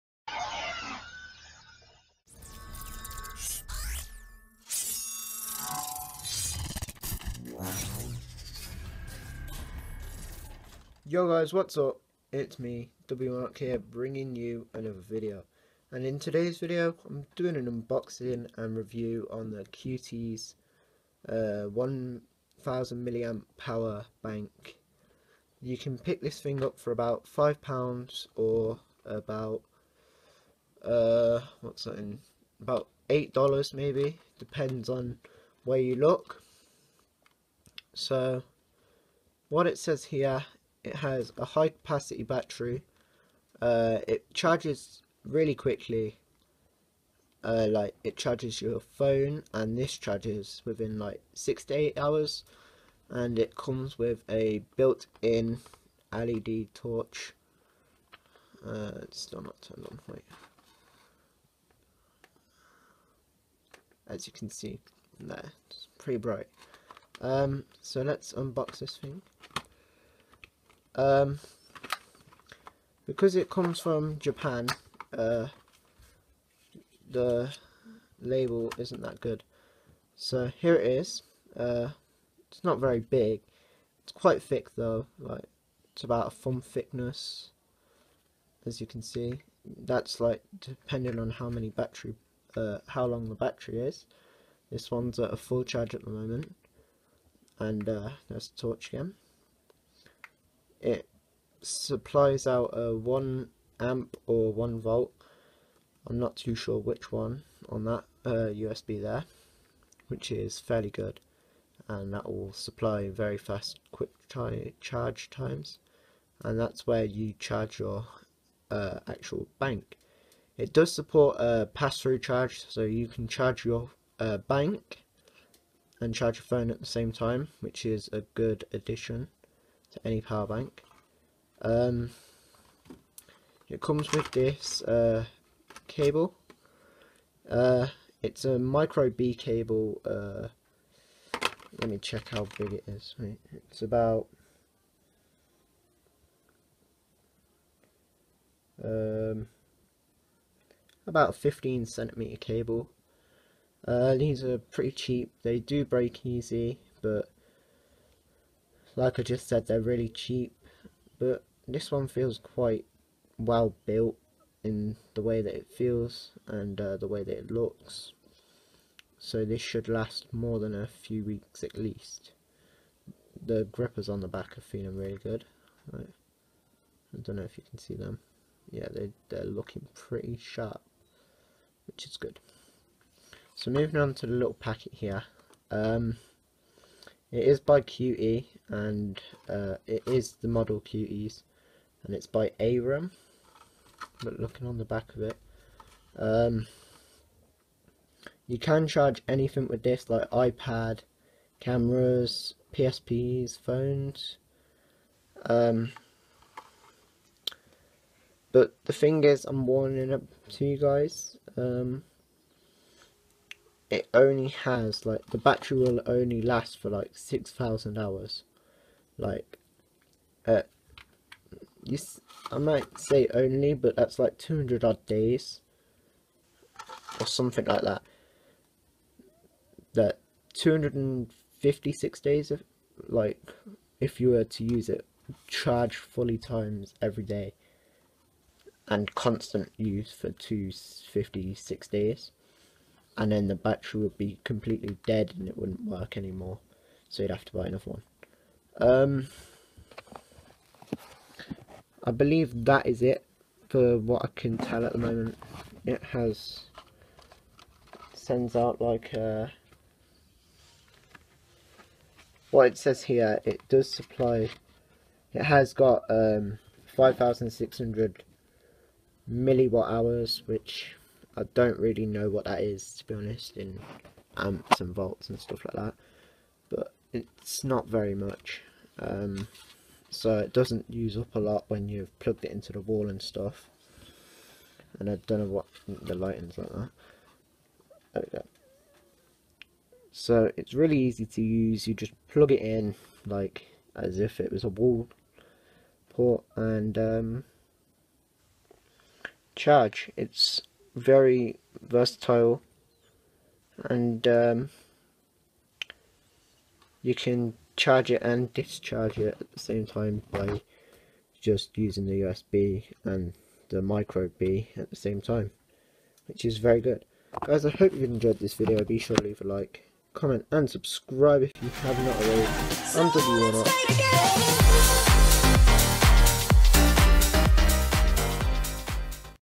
ARUN cuties 5600mwh power bank unboxing review
this is a review of the cuties 1000mA power bank it has a torch and is able to out put 5.0/1A this is good for if you need just a quick charge before your phone dies and you need to phone someone.
wanna know where to get one? here the
intro was made by  Theminecraftking106
they make amazing controllers and are very high quality